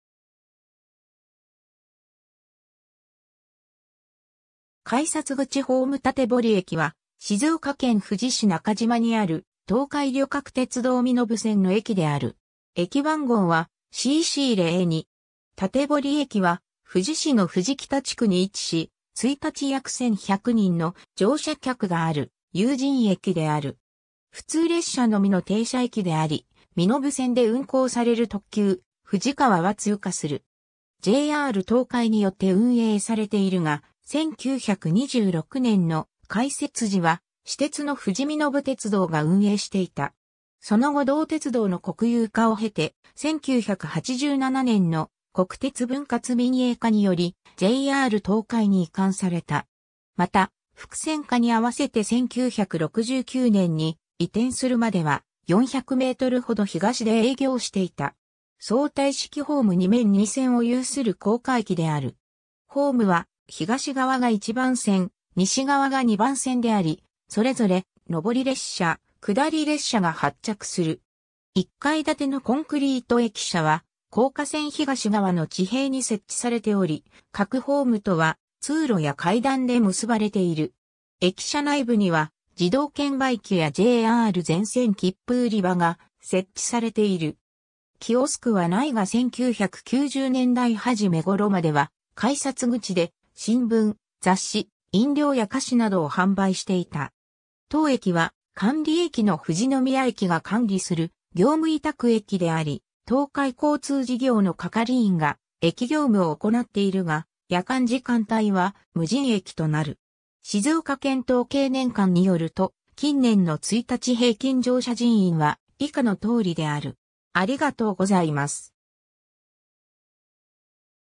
竪堀駅
改札口（2018年4月） ホーム（2007年10月） 竪堀駅（たてぼりえき）は、静岡県富士市中島にある、東海旅客鉄道（JR東海）身延線の駅である。
駅番号はCC02。
 竪堀駅は富士市の富士北地区に位置し、1日約1100人の乗車客がある有人駅である。
普通列車のみの停車駅であり、身延線で運行される特急「ふじかわ」は通過する。
 JR東海によって運営されているが、1926年（大正15年）の開設時は私鉄の富士身延鉄道が運営していた。
その後同鉄道の国有化を経て、1987年（昭和62年）の国鉄分割民営化によりJR東海に移管された。
また、複線化にあわせて1969年（昭和44年）に移転するまでは、400メートルほど東で営業していた。
 相対式ホーム2面2線を有する高架駅である。
ホームは東側が1番線、西側が2番線であり、それぞれ上り列車、下り列車が発着する。
 1階建てのコンクリート駅舎は高架線東側（1番線側）の地平に設置されており、各ホームとは通路や階段で結ばれている。
駅舎内部には自動券売機やJR全線きっぷうりばが設置されている。
キオスクはないが1990年代はじめ頃までは改札口で新聞、雑誌、飲料や菓子などを販売していた。
 当駅は管理駅の富士宮駅が管理する業務委託駅であり、東海交通事業の係員が駅業務を行っているが、夜間時間帯は無人駅となる。
 「静岡県統計年鑑」によると、近年の1日平均乗車人員は以下の通りである。